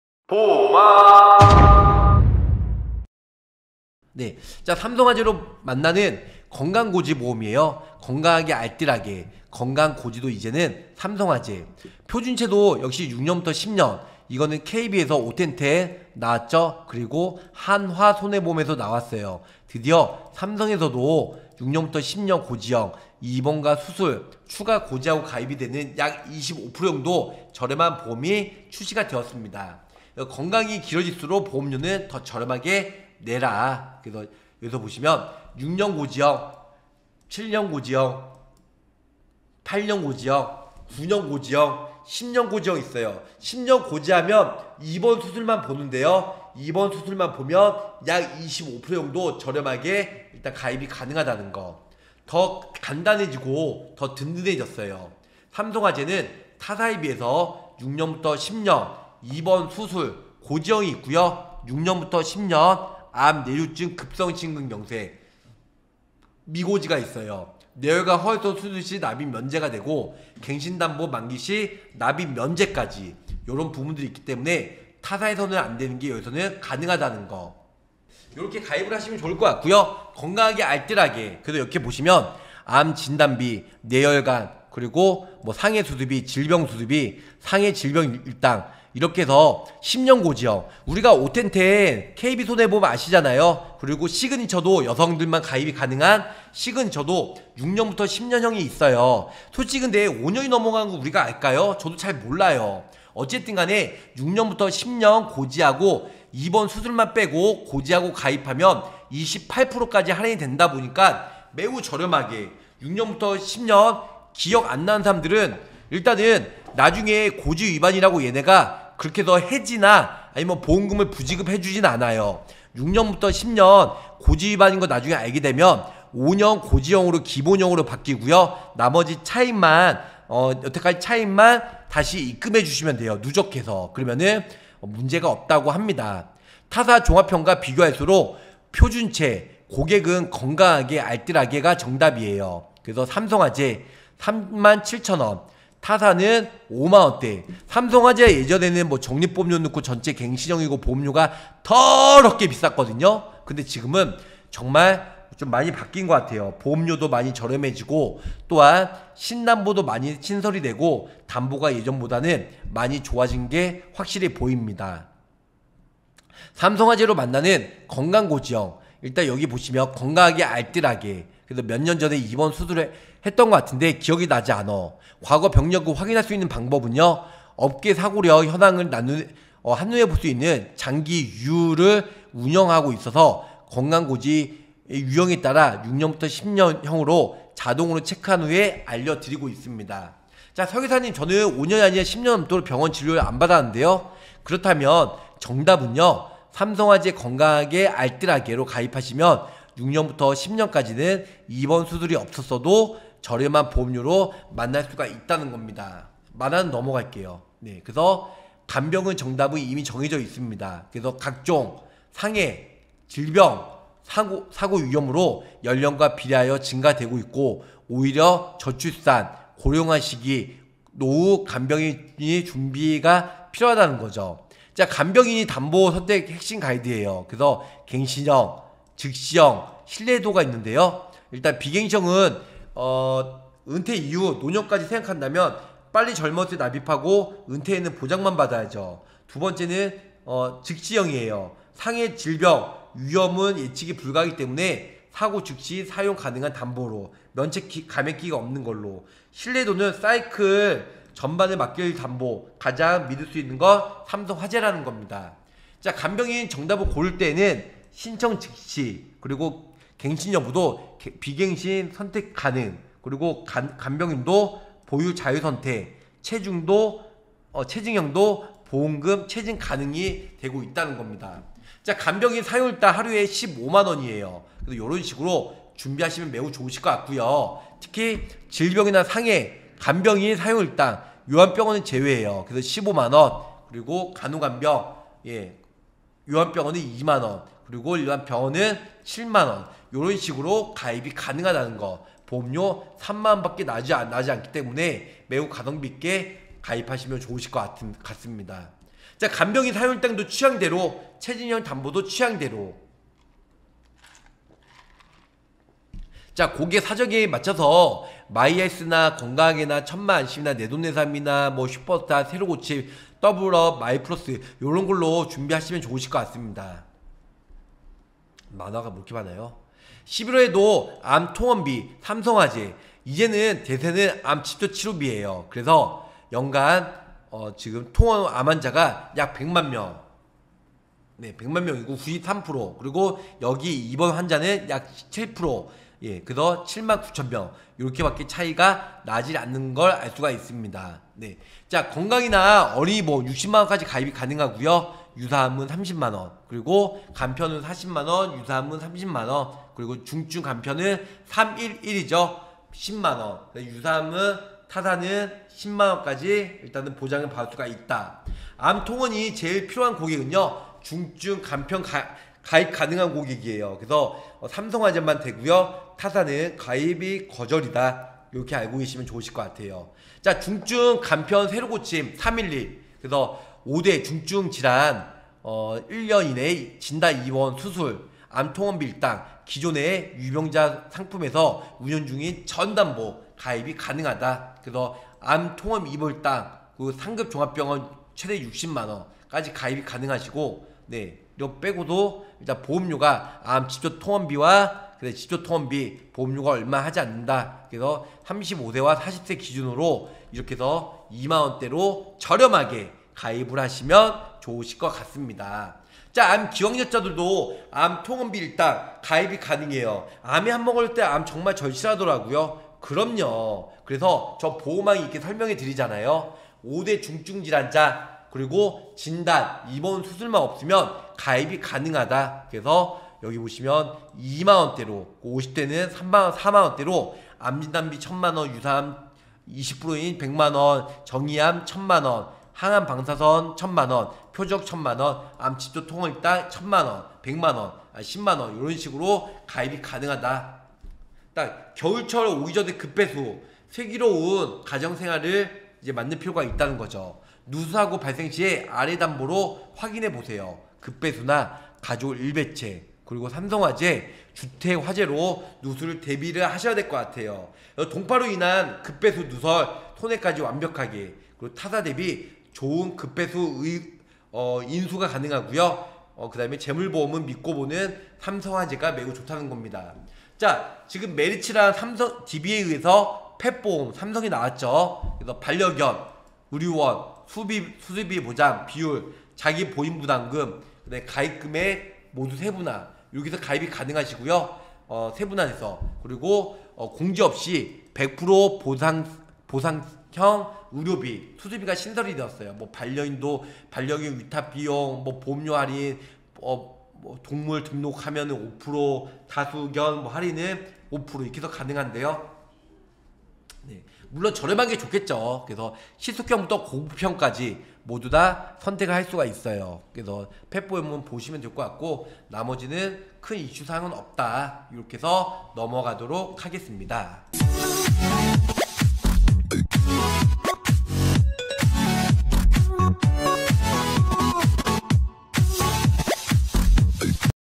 11월 삼성화재 신상품안내!! (10년고지 28%할인)
안녕하세요~~

삼성화재 신상품을 소개합니다
28%할인된  보험으로 추천합니다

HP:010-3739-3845
카톡친구추가:r2an
카톡플러스친구:보험왕검색